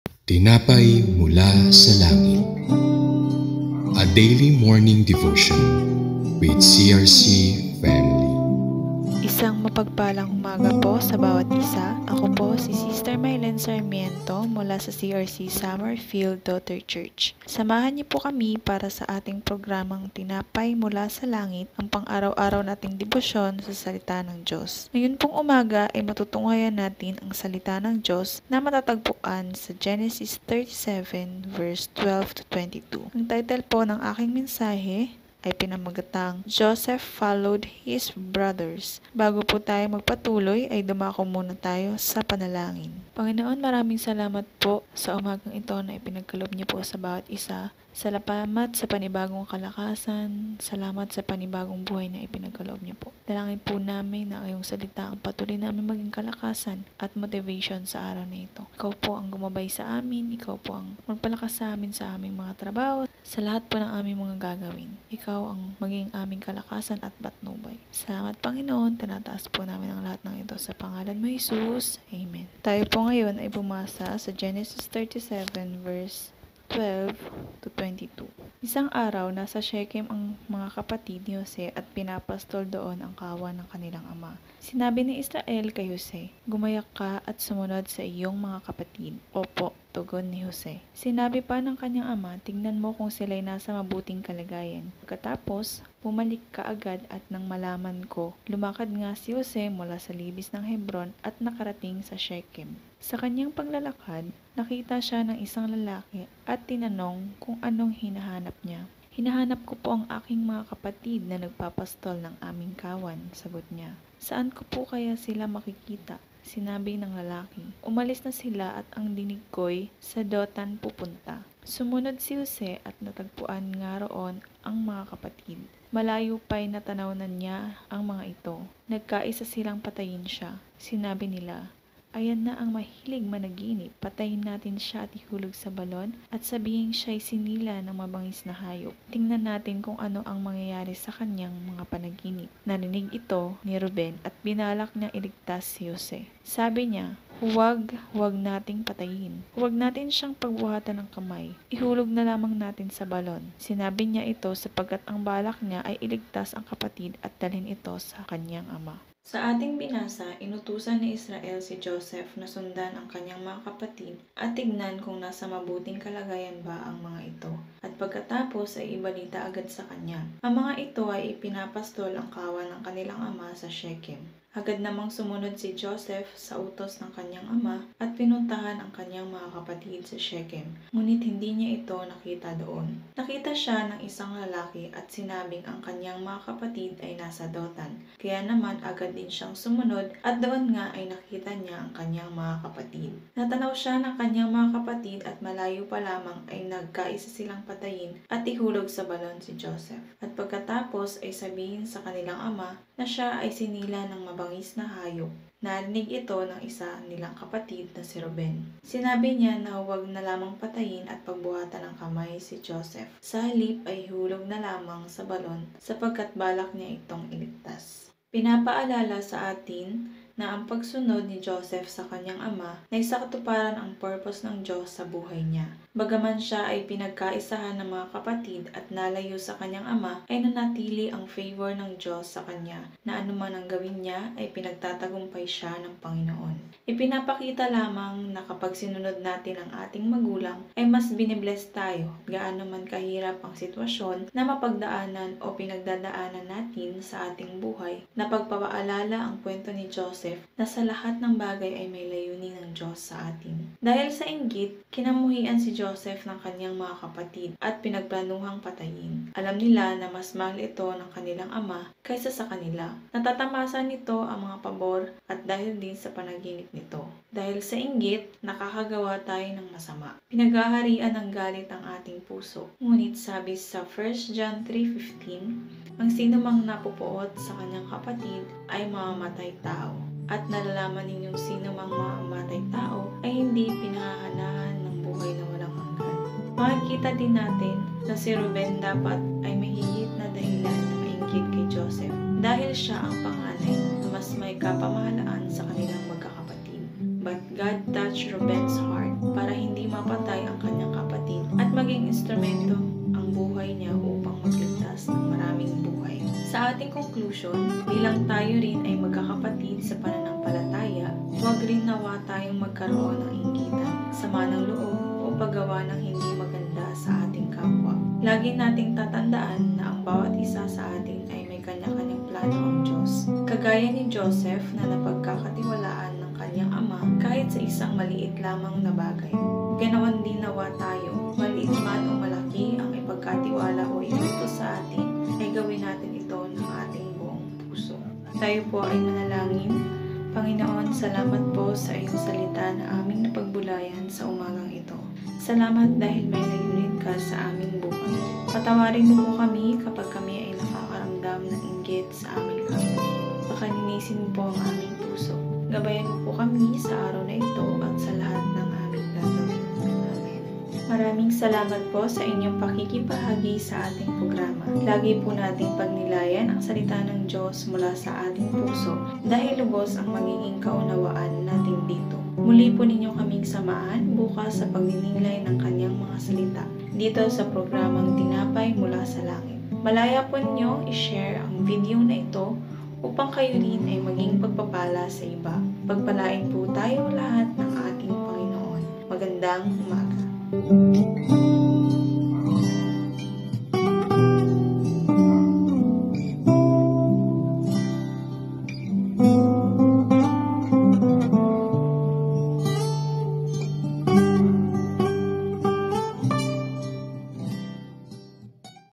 Tinapay Mula Sa Langit: Episode 173 ~ Joseph Followed His Brothers (Genesis 37:12-22)
For your prayer needs, you can message us through our FB